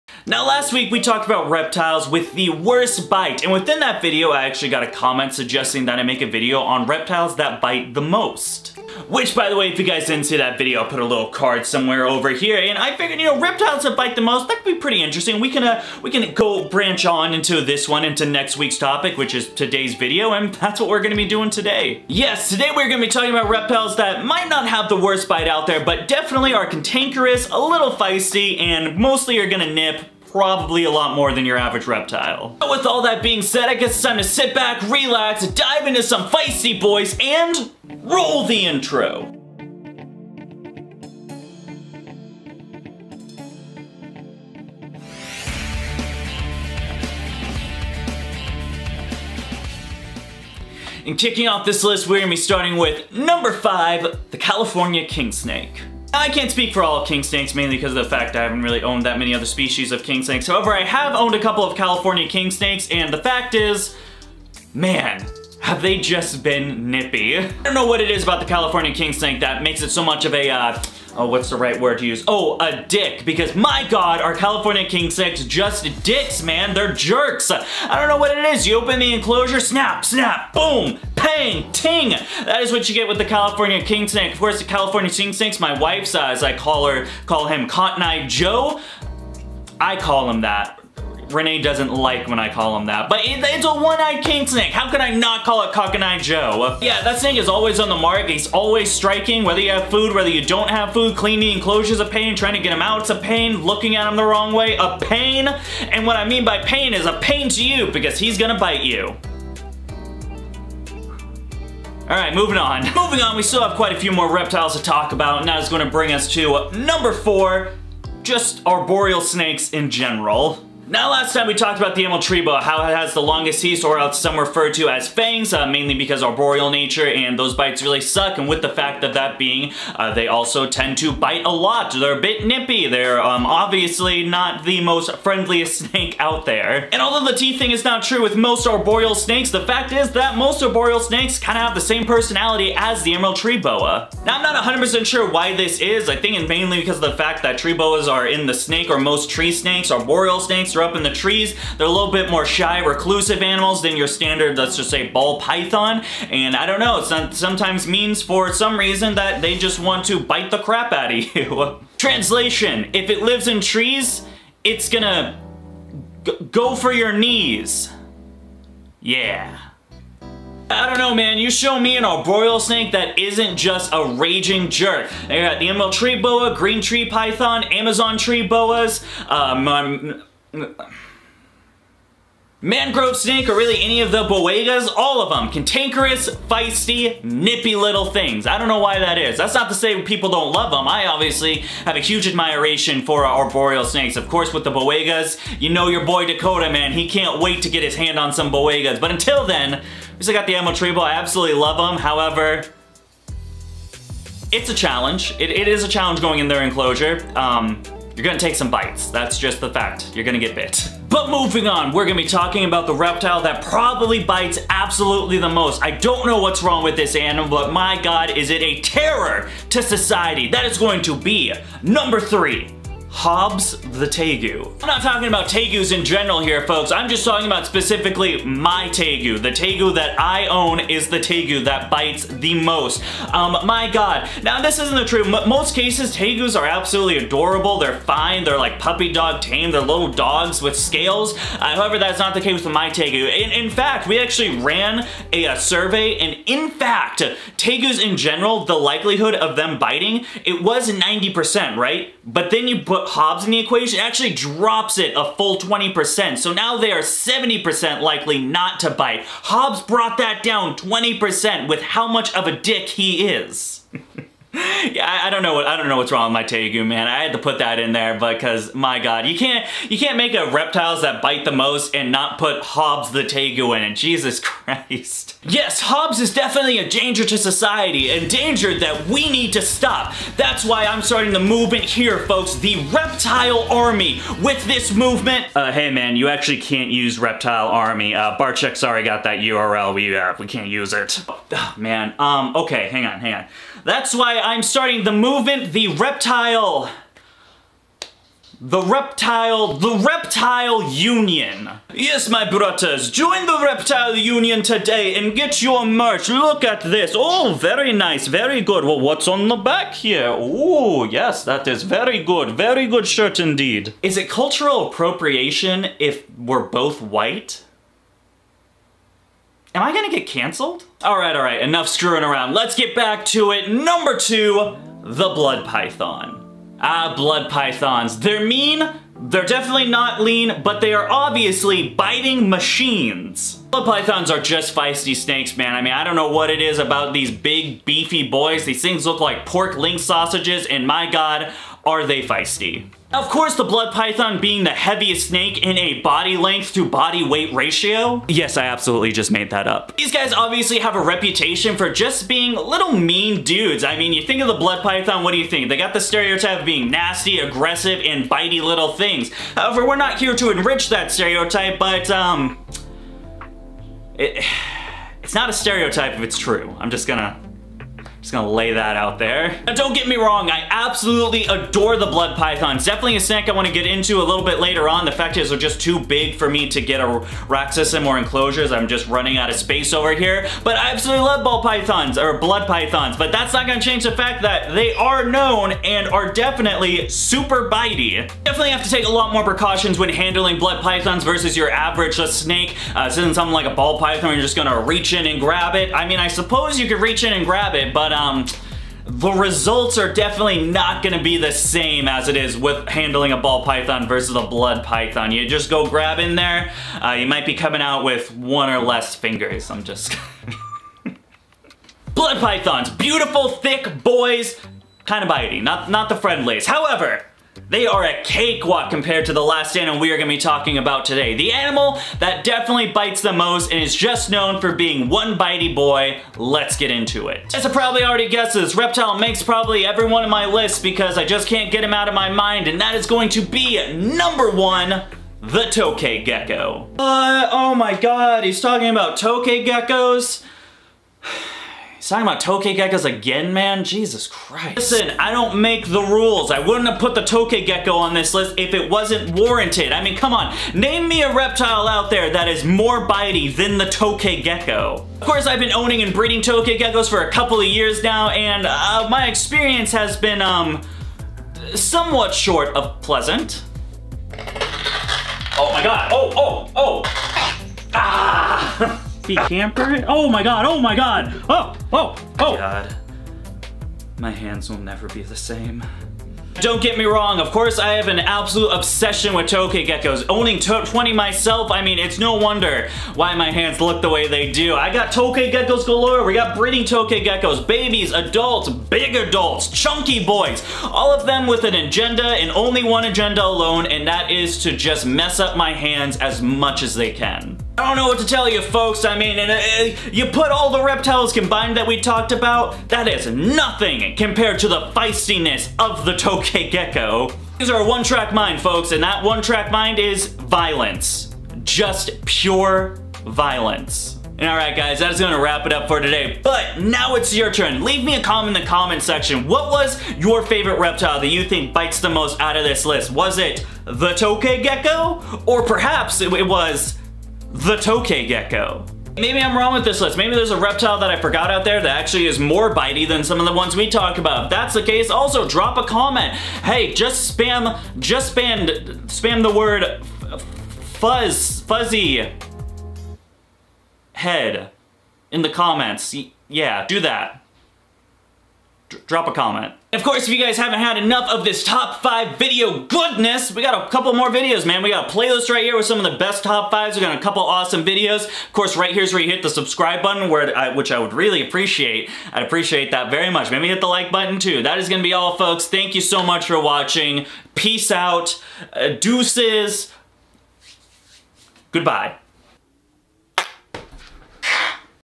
Top 5 Reptiles That Are MOST Likely To Bite
in this video we go over the top 5 reptiles that are most likely to bite. we talk about some reptiles with spunky personalities that can be a little more fresh then your average reptile. although these reptiles are more prone to bite that is not to say they are in any way a bad reptile. infact i keep and love the majority of reptiles that are on this list.